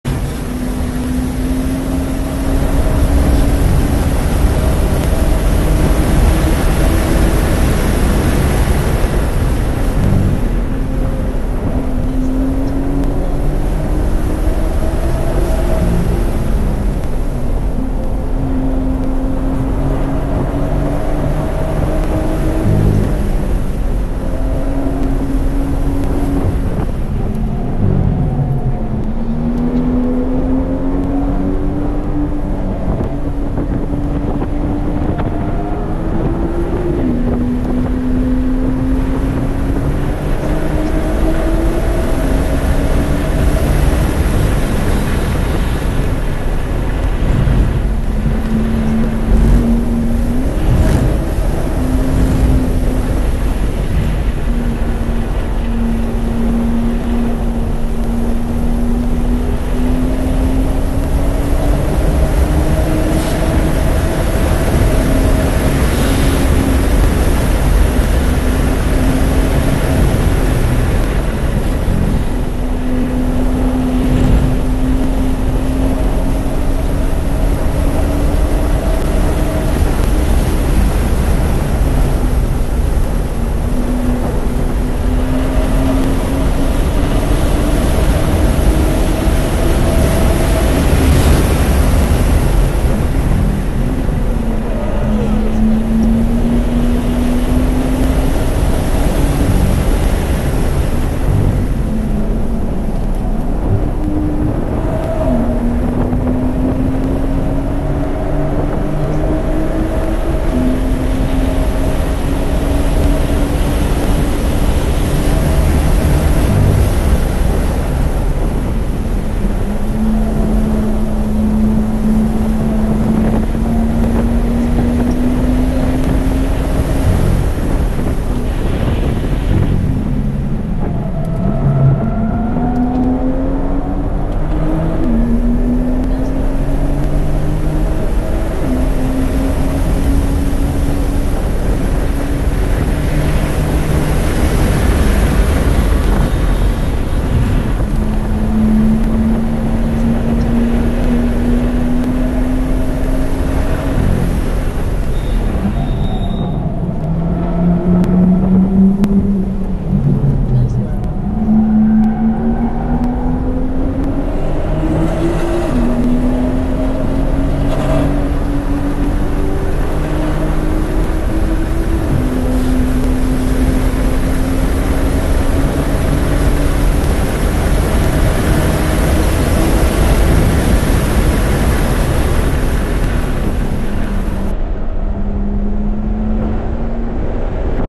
Jeff ride along OIR Spring 23 HD 1080p
When you have someone willing to trust you at speed, it is easier to share what's really happening in the car.  Otherwise, things look smooth with cameras attached to the car and a driver who is harnessed in the seat.